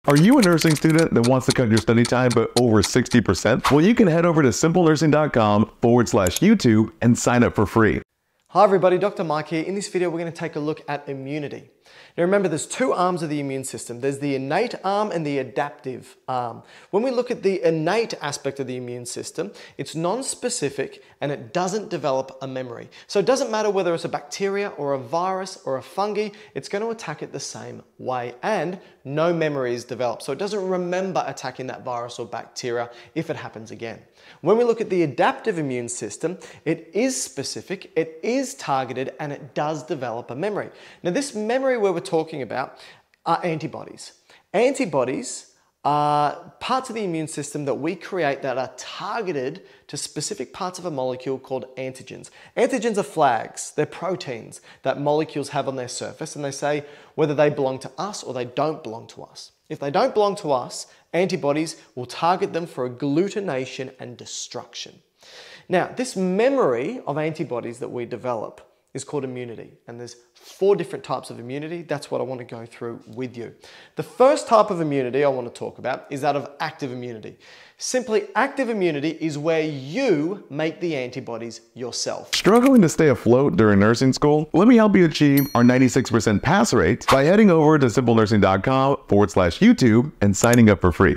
Unlock Immunity Concepts: Essential Guide for Nursing Students
For the NCLEX:

... and more!

Dive deep into the complexities of immunity with our comprehensive guide tailored for nursing students preparing for the NCLEX exam. This video delves into the intricacies of the immune system, covering key concepts and mechanisms essential for success in nursing practice and exam performance. From understanding the role of antibodies to mastering immune response pathways, we provide clear explanations and practical insights to boost your confidence. With a focus on NCLEX-relevant content, we help you navigate through challenging topics, ensuring you're well-equipped to excel in your exams and future clinical endeavors. Don't miss out on this essential resource designed to elevate your understanding of immunity and ace the NCLEX!